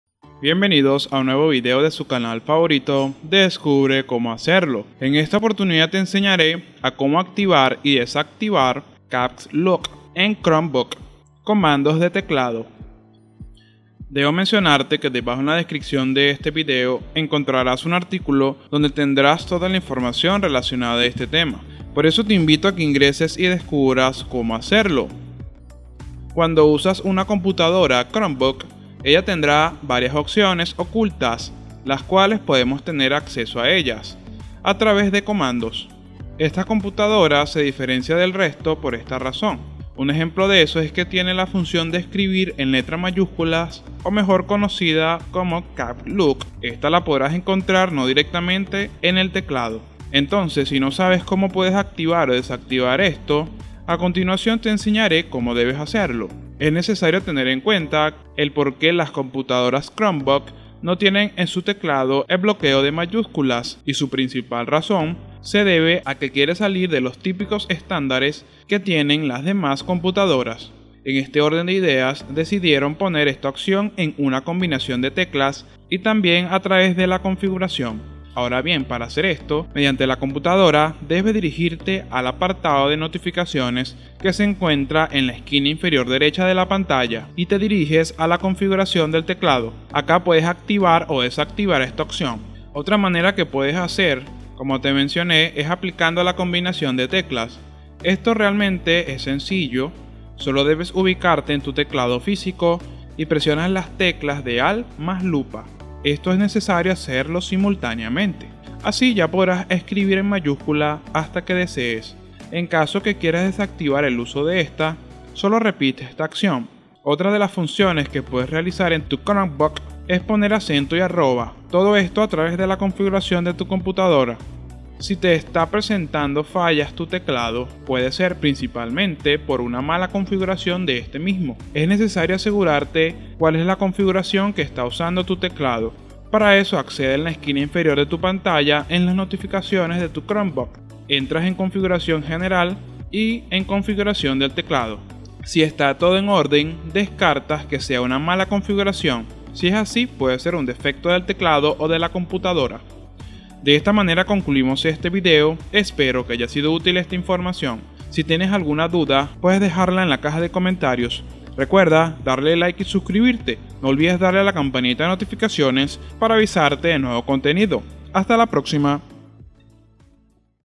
¿Cómo Activar y Desactivar 'Caps Lock' en Chromebook? - Comandos de Teclado
En este video descubre ¿Cómo Activar y Desactivar 'Caps Lock' en Chromebook? - Comandos de Teclado. En el Canal Descubre Cómo Hacerlo encontrarás el mejor contenido. ¡No te lo puedes perder!